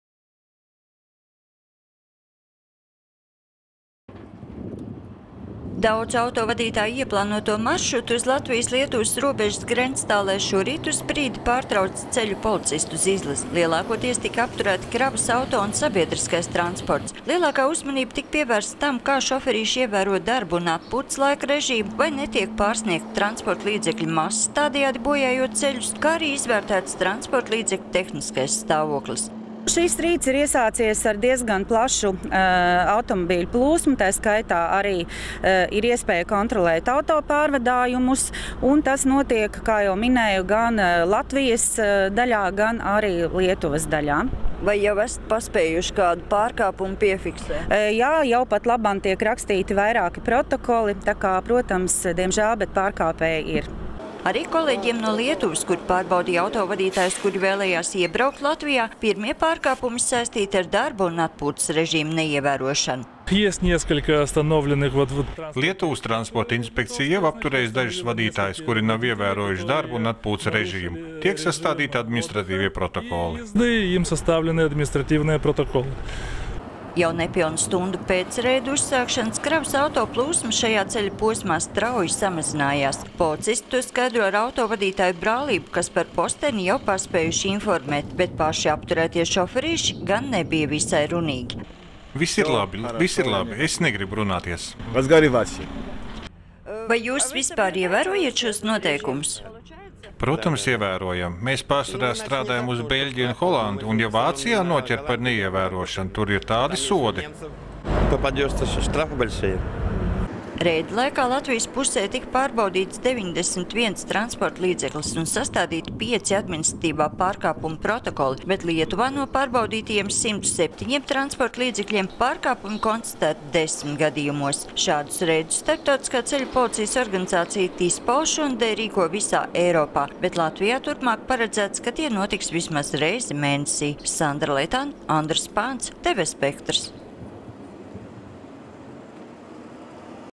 Grenctālē reida laikā pārbauda kravas automašīnas
Lielu pārsteigumu šorīt nevienam vien  auto vadītājam uz Latvijas-Lietuvas robežas sagādāja ceļu policija. Reidā, kas vienlaikus notika abās kaimiņvalstīs, lielākā uzmanība tika pievērsta tam, vai  kravas auto šoferīši ievēro darba un atpūtas režīmu, kā arī transporta līdzekļu tehniskajam stāvoklim.